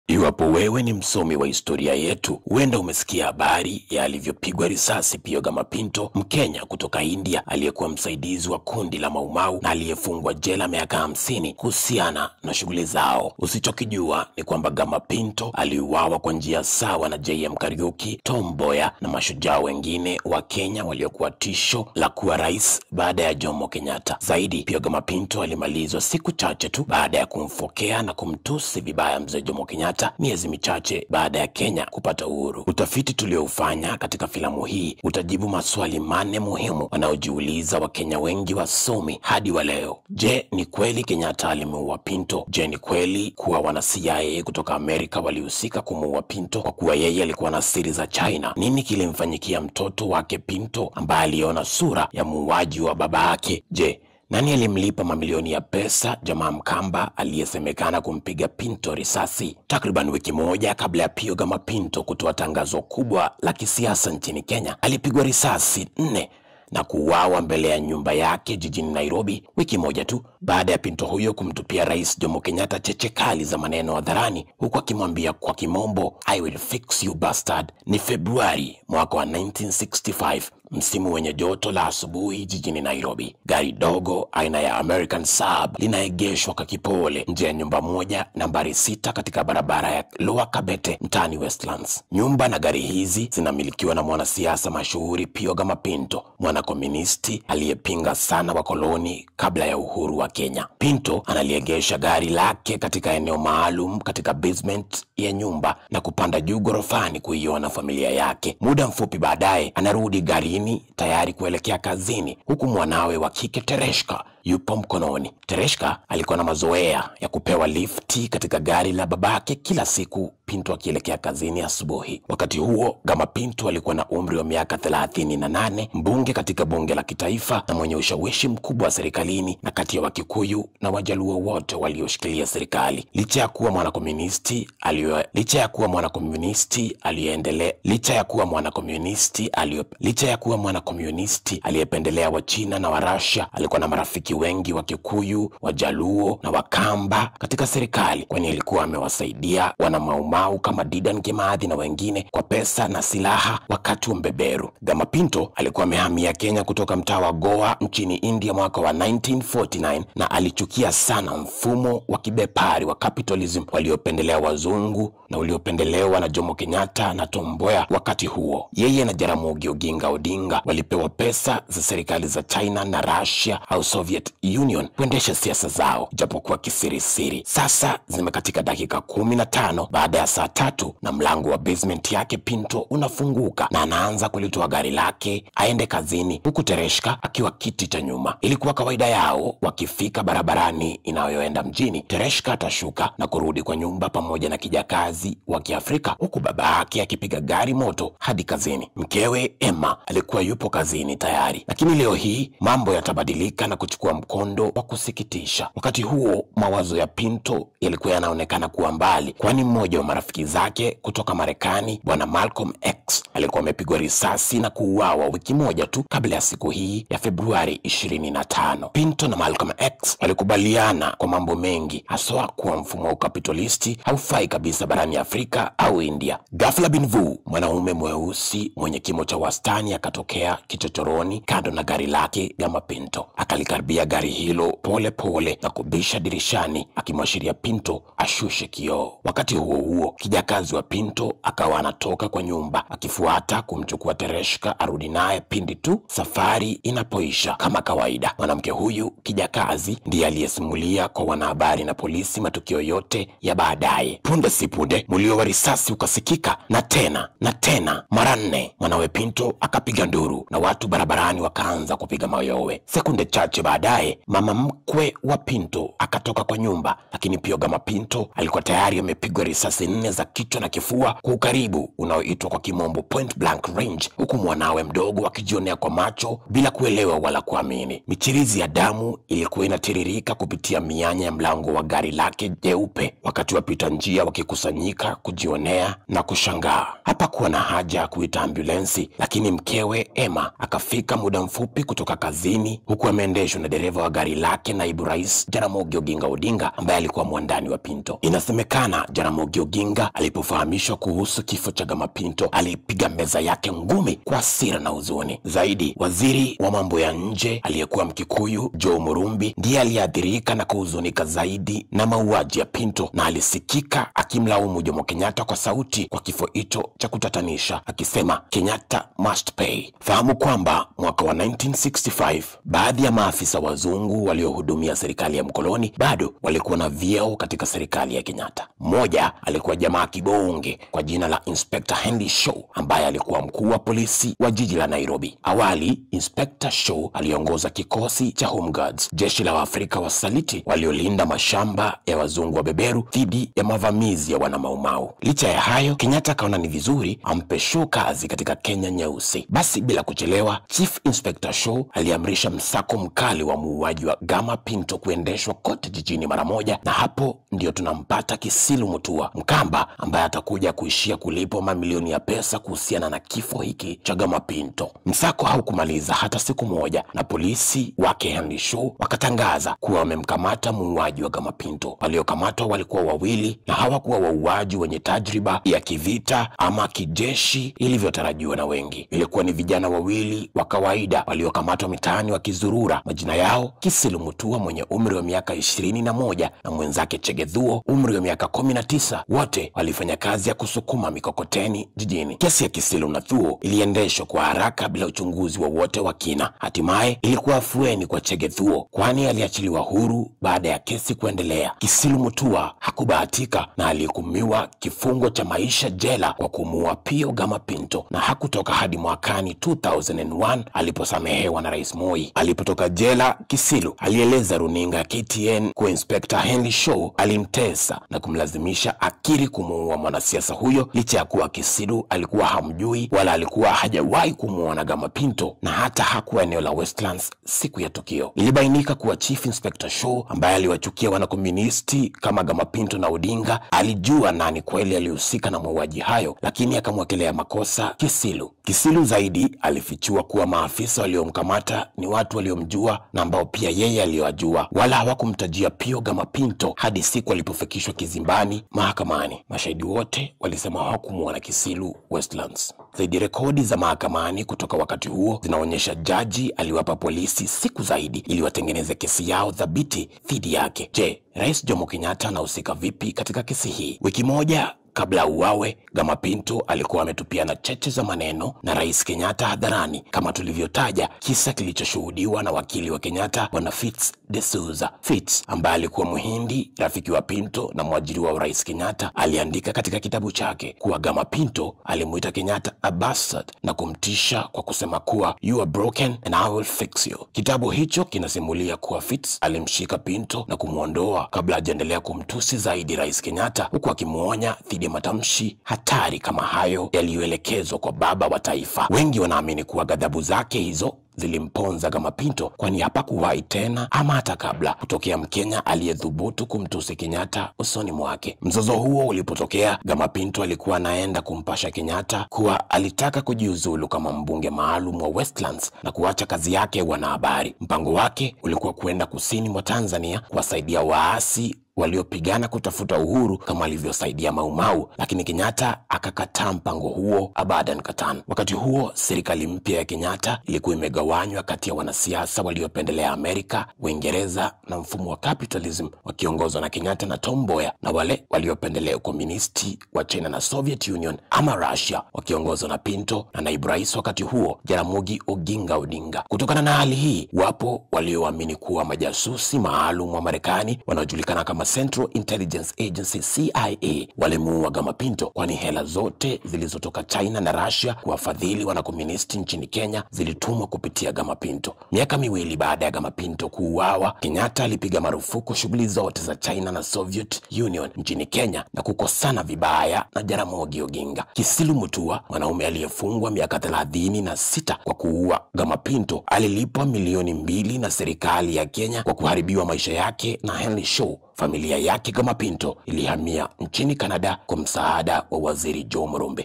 Gama Pinto: RISASI Mbele ya Mwanawe
STORY BOOK :Katika Historia ya Kenya hii leo, tunaangazia story yake shujaa Pio Gama Pinto aliyekuwa rafiki wa MauMau na aliyeuawa Kwa Unyama miaka ya 1960s baada ya kenya kuapata uhuru kutoka kwa mzungu mkoloni.
Rais Mwanzilishi Mzee Jomo Kenyatta, CIA na wengine wamehusishwa na mauaji ya Pinto, na filamu hii inafichua usiyoyajua kuhusu siku Alipouawa kinyama nyumbani kwake westlands, nairobi.